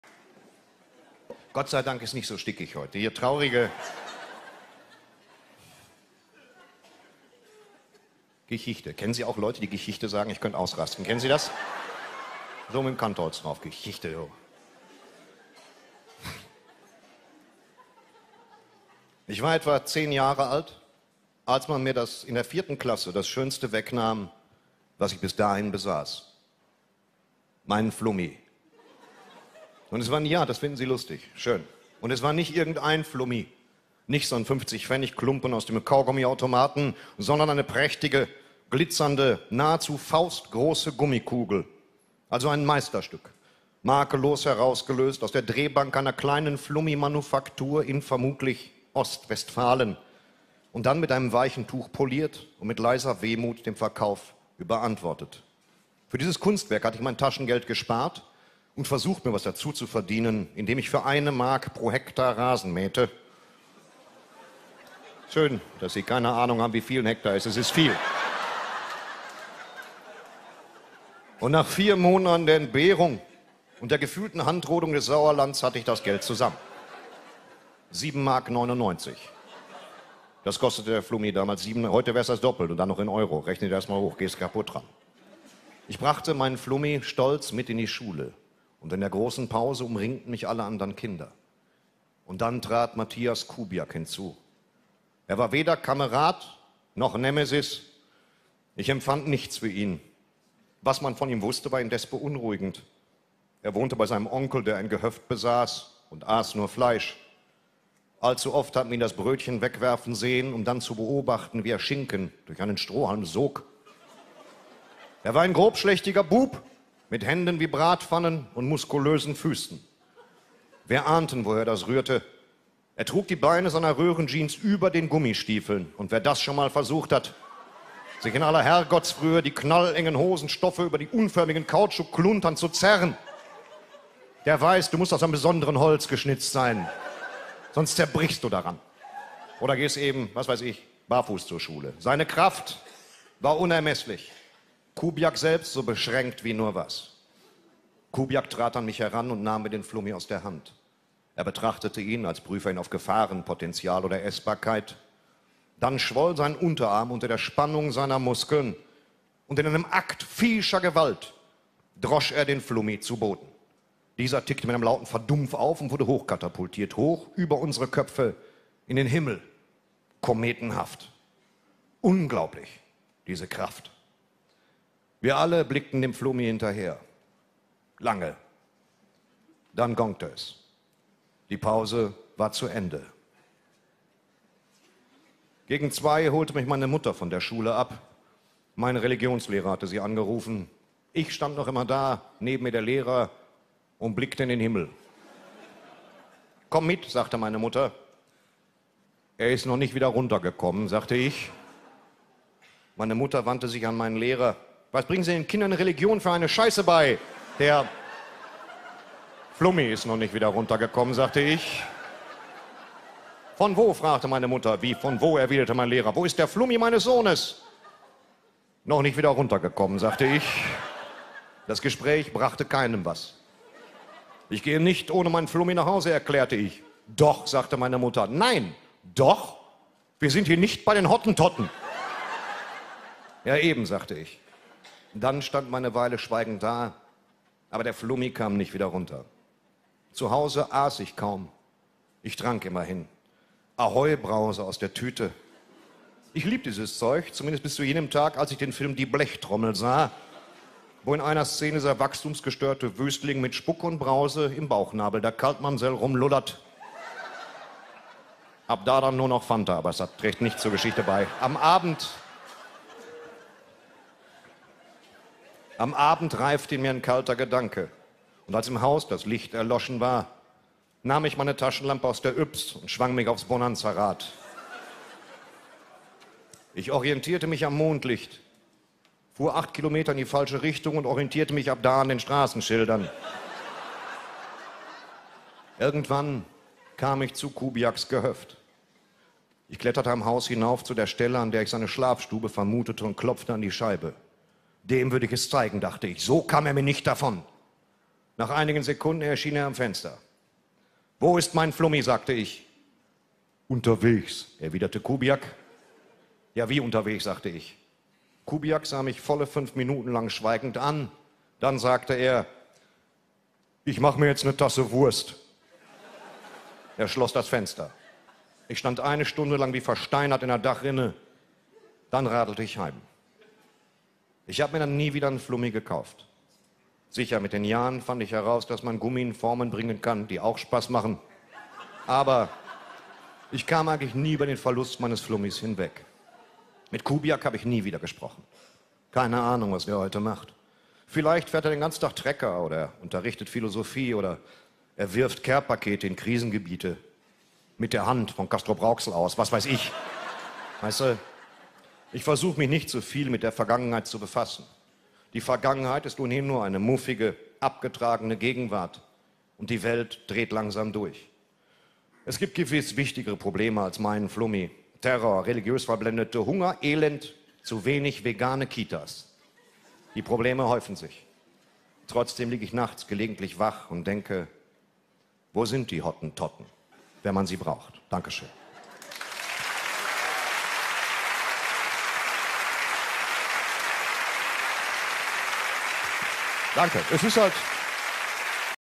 Torsten Sträter Teil II DER FLUMMI, Matthias Kubiak, AHOI Brause und Hottentotten
Wer ist Matthias Kubiak und was hat er mit dem Flummi gemacht? Die AHOI Brause, Wo sind die Hottentotten, wenn man sie braucht?